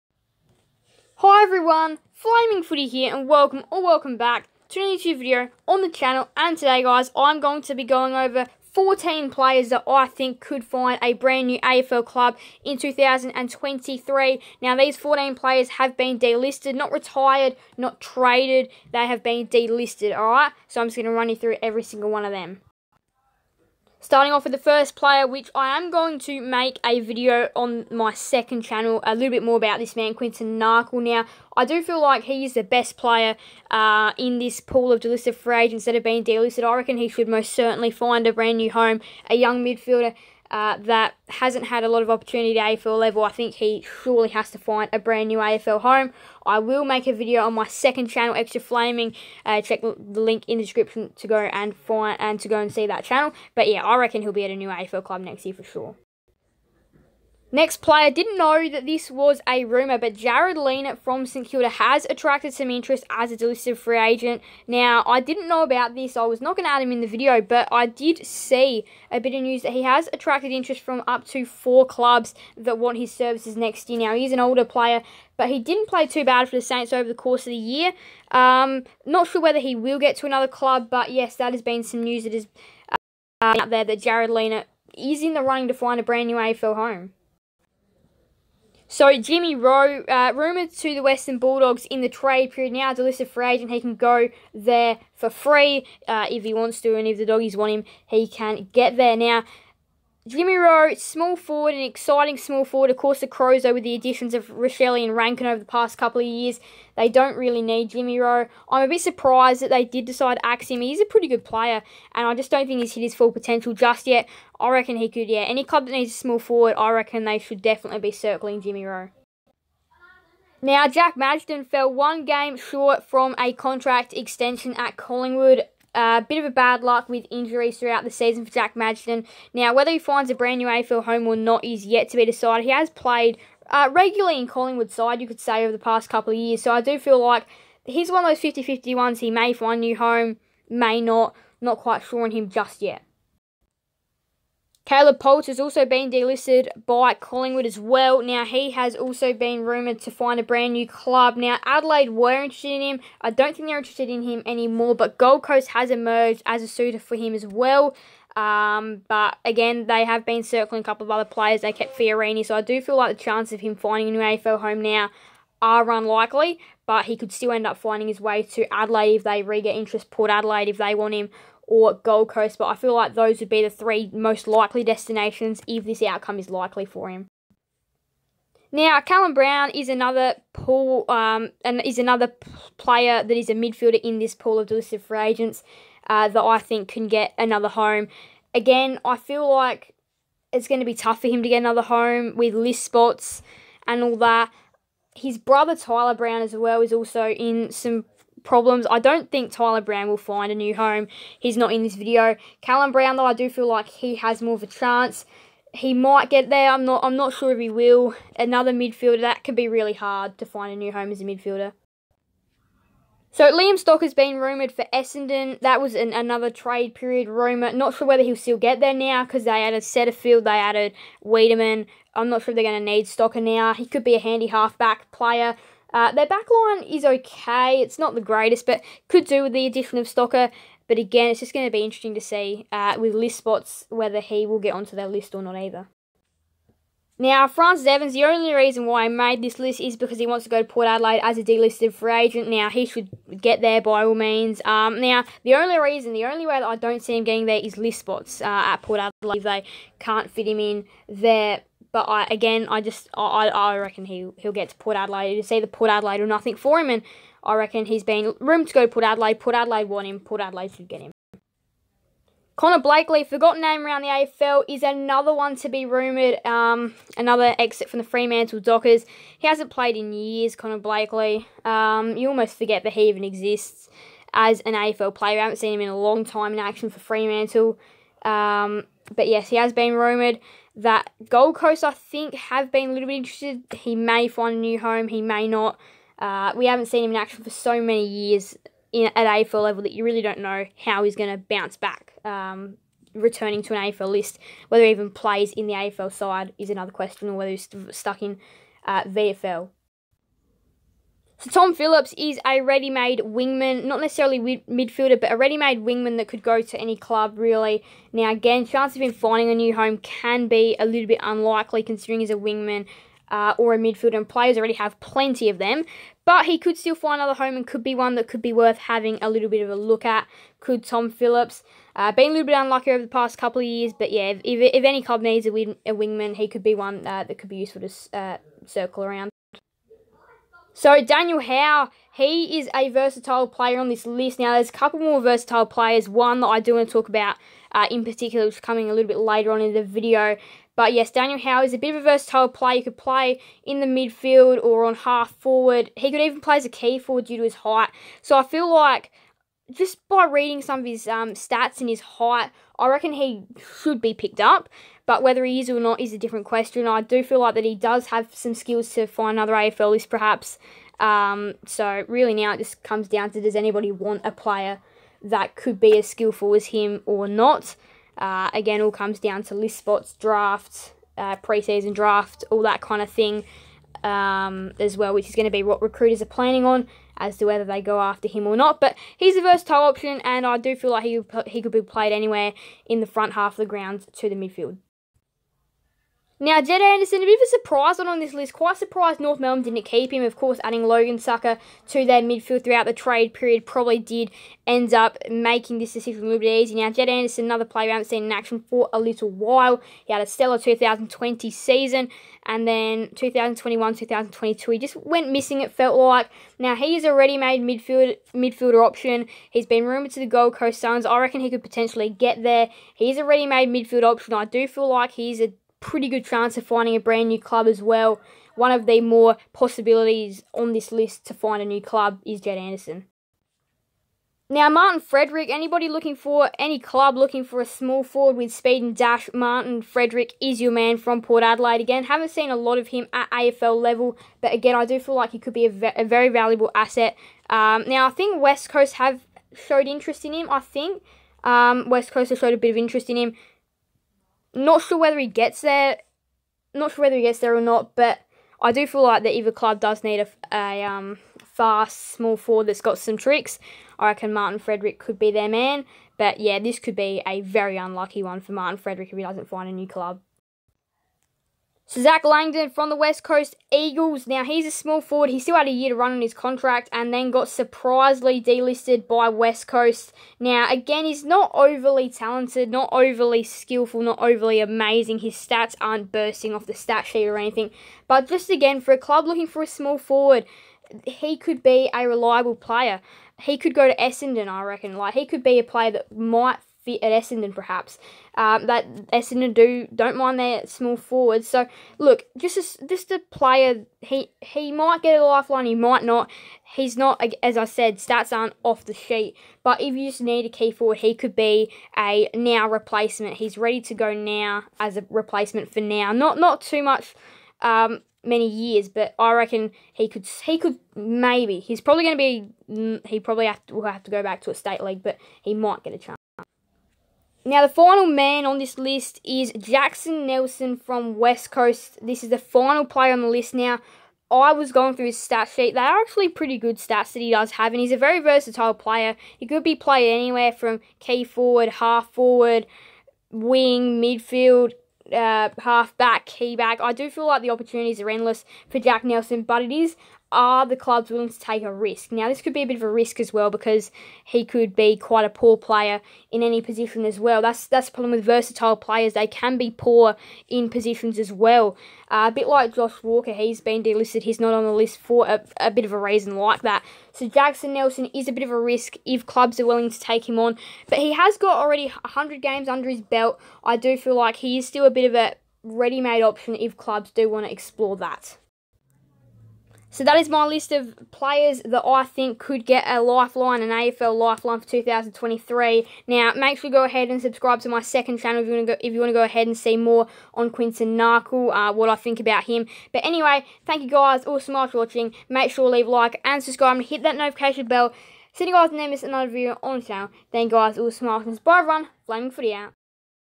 Delisted Players That Could Find New AFL Clubs!
In todays video I share with you all the players in the delisted free agency period that have a decent chance of finding a new AFL Club for 2023!